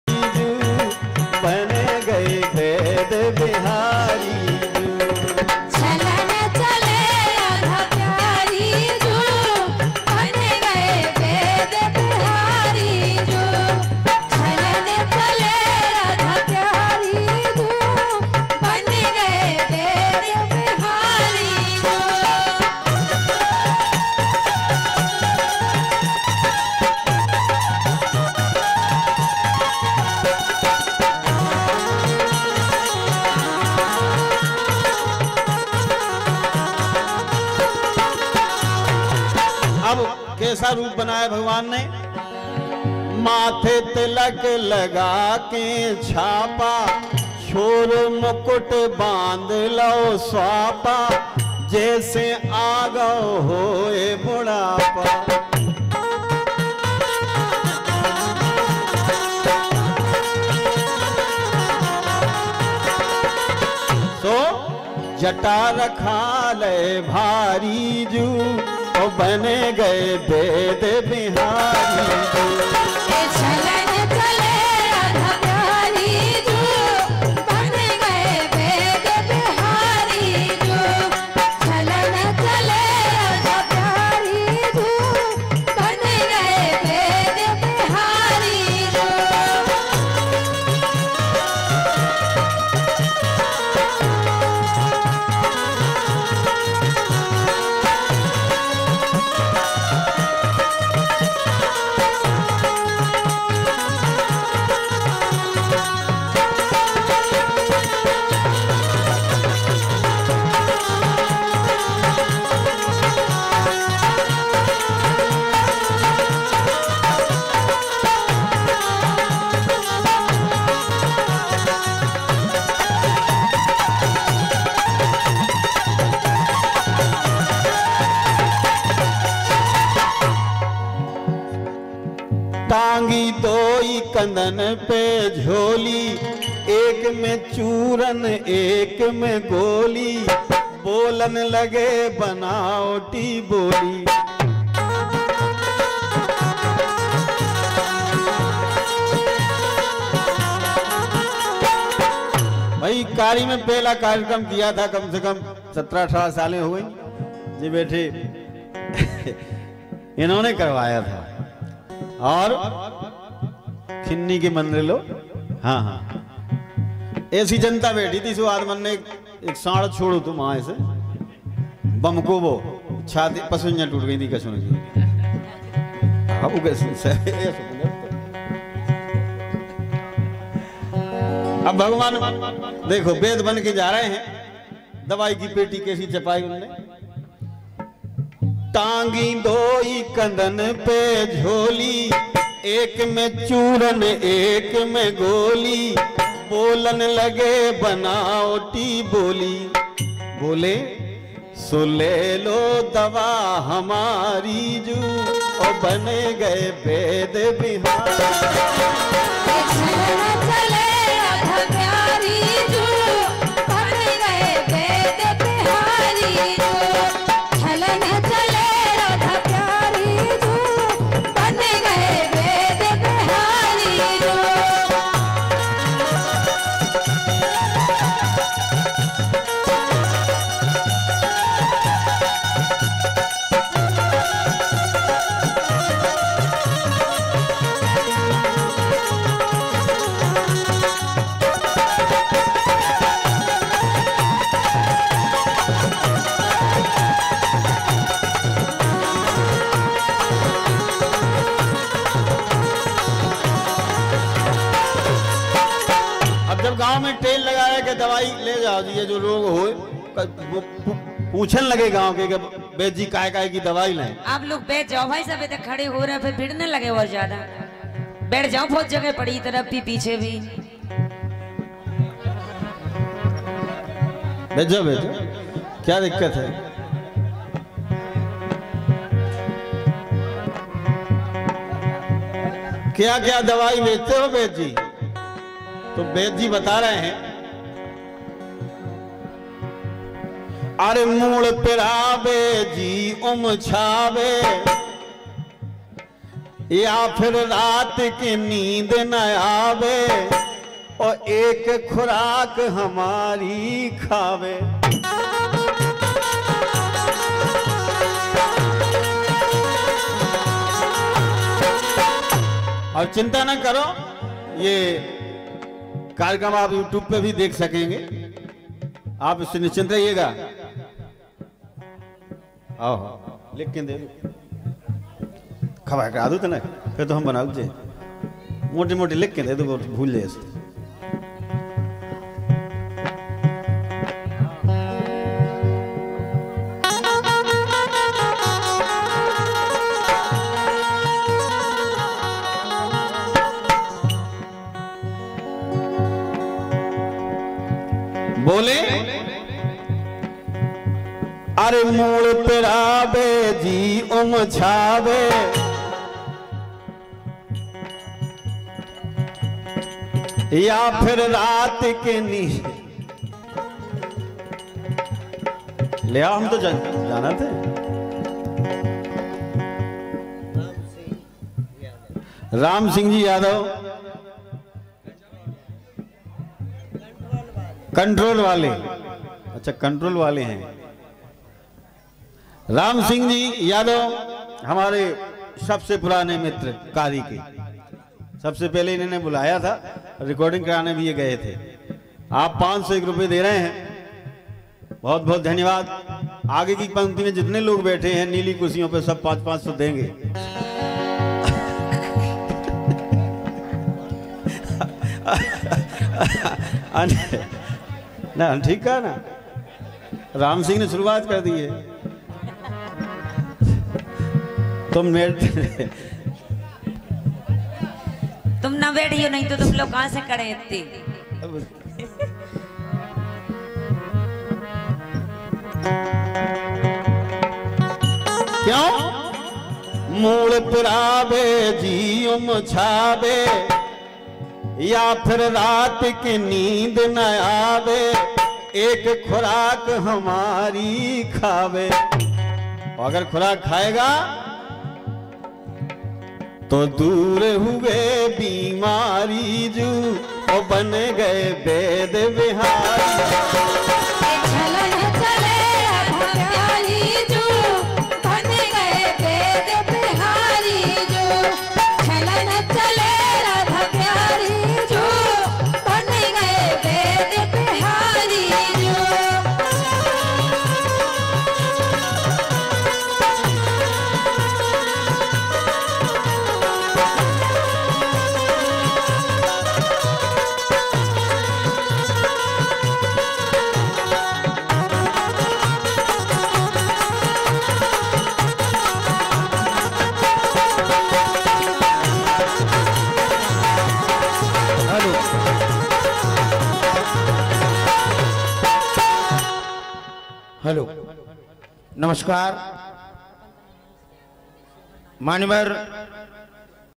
बलराम यादव एवं रश्मि यादव का रंगारंग प्रोग्राम कारी
new program  कारी राकेश   गिरी के सम्मान में जवाबी प्रोग्राम बलराम एवं रश्मि यादव